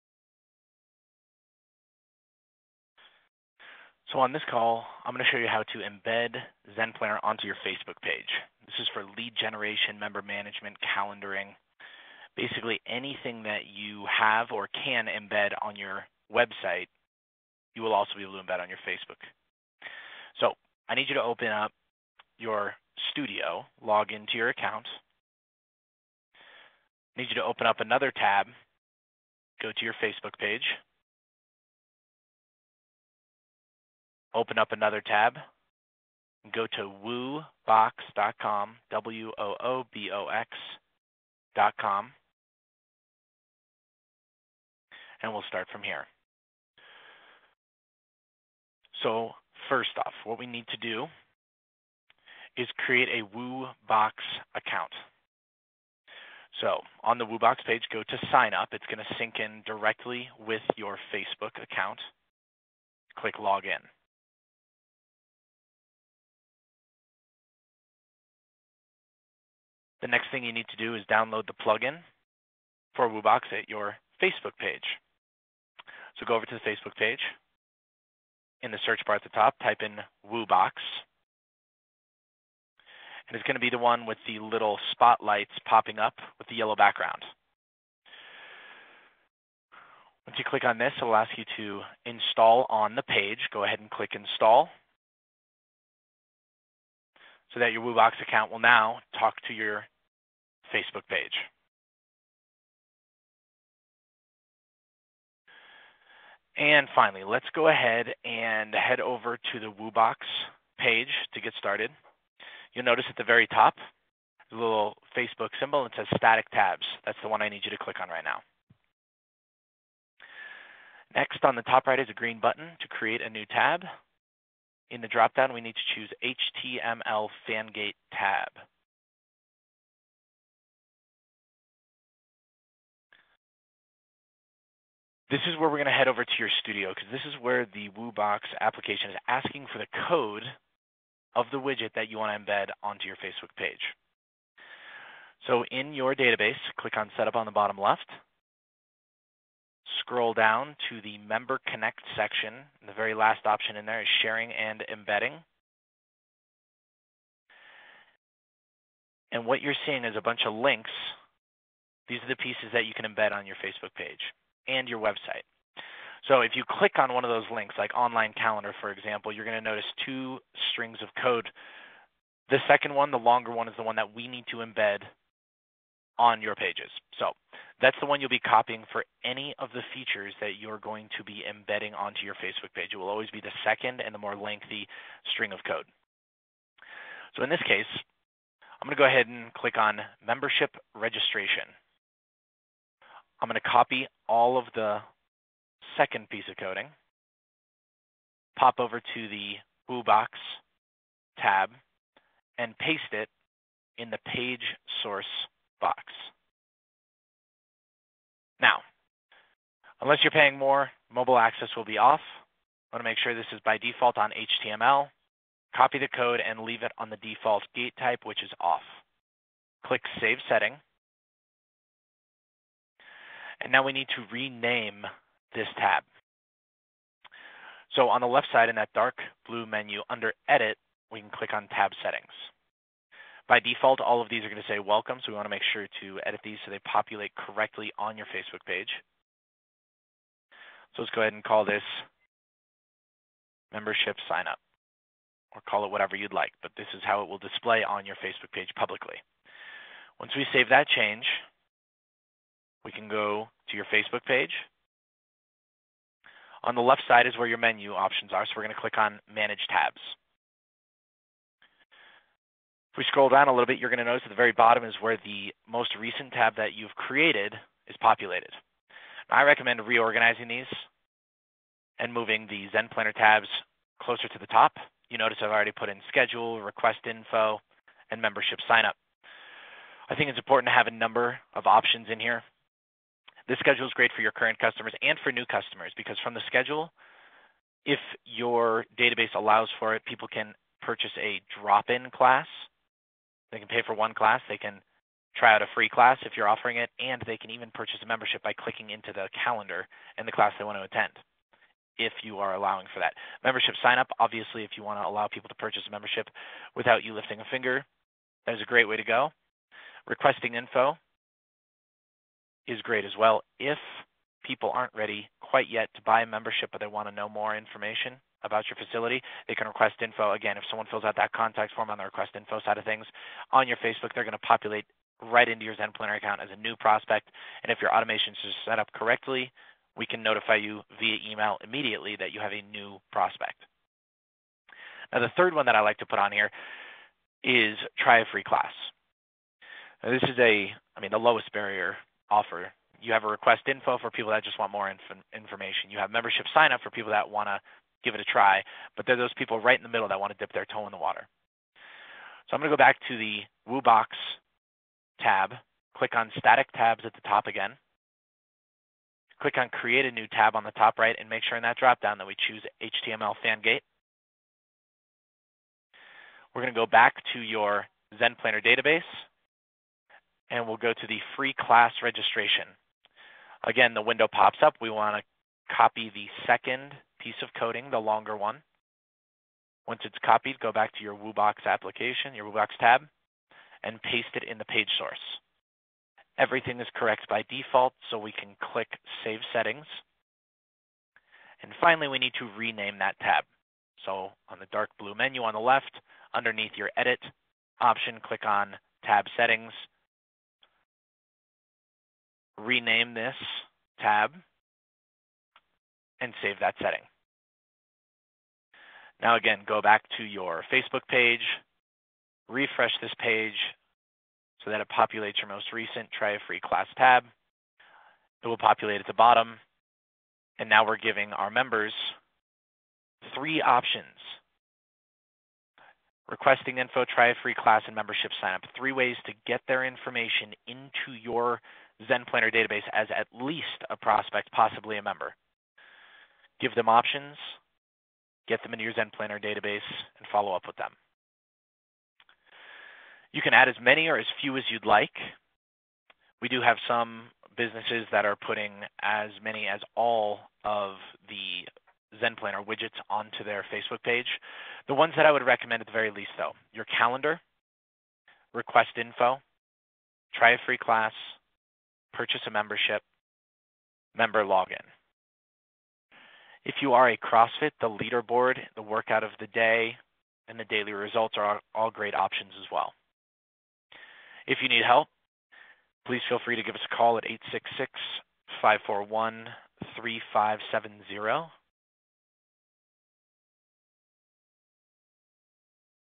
Embedding Zen Planner on your Facebook Page
This video shows how to embed your Zen Planner iFrames on your Facebook page